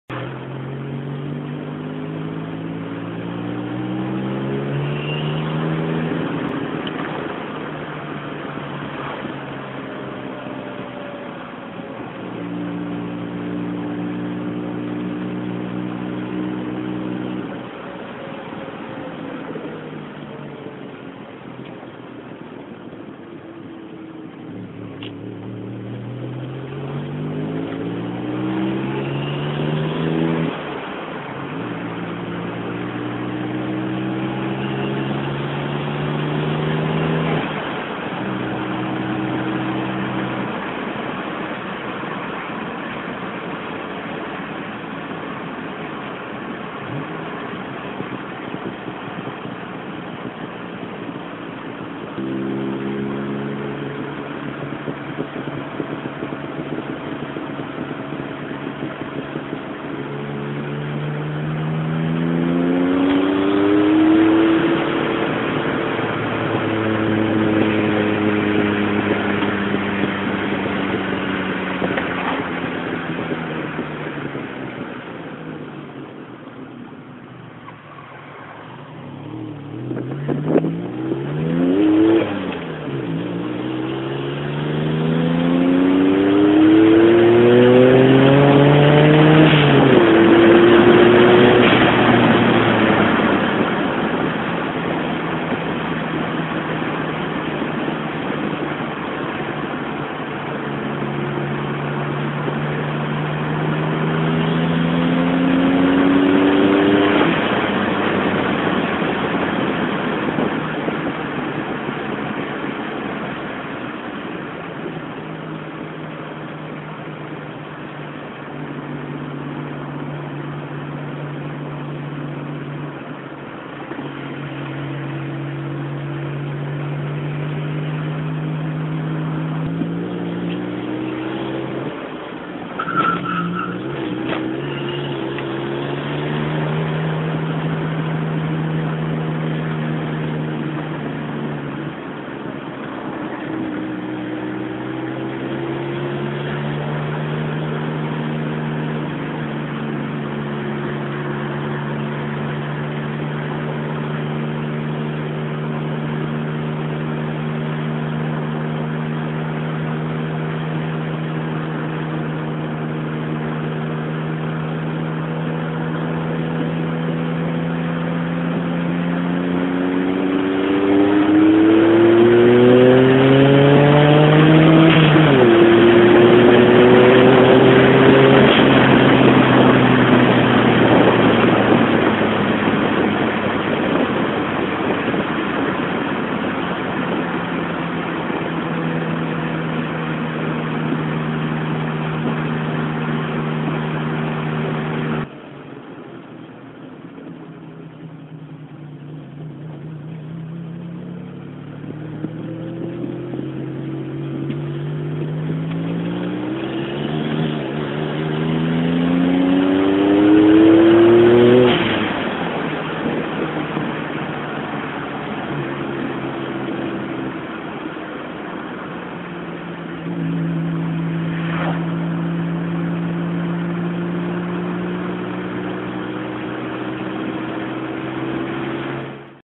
Capri Testfahrt
Kurze Testfahrt vor der Klassikwelt Bodensee mit notdürftig instandgesetztem Lader. Läuft leider noch nicht optimal, aber mehr war in der kurzen Zeit nicht zu machen.